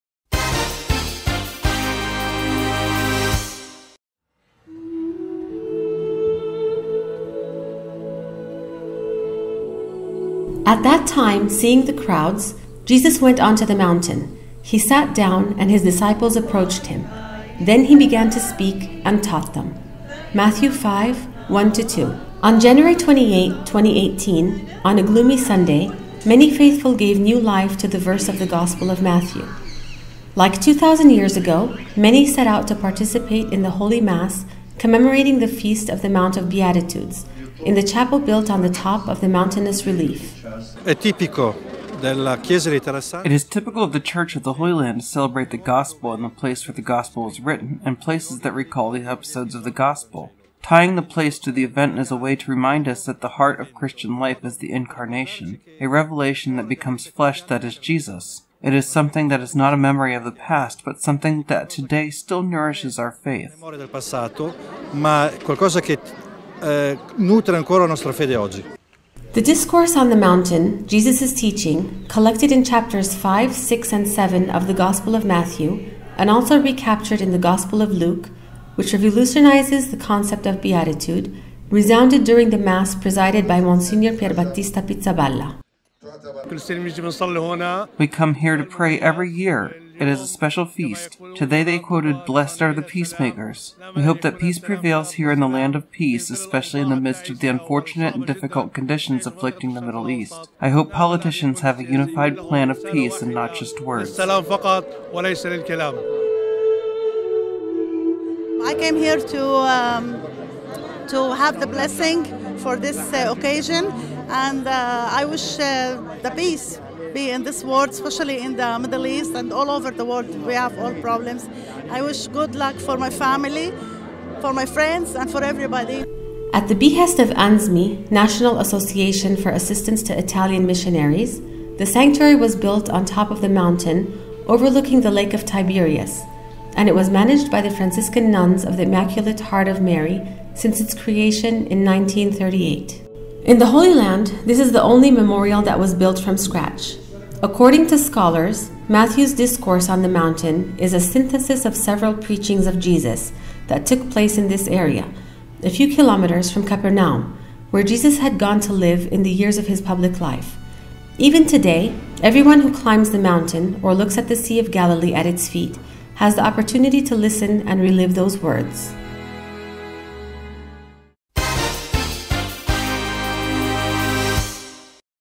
On the Mount of Beatitudes to relive the Gospel
On January 28, the Church celebrates the festival of the mountain overlooking the lake of Tiberias and remembers Jesus' Sermon on the Mount.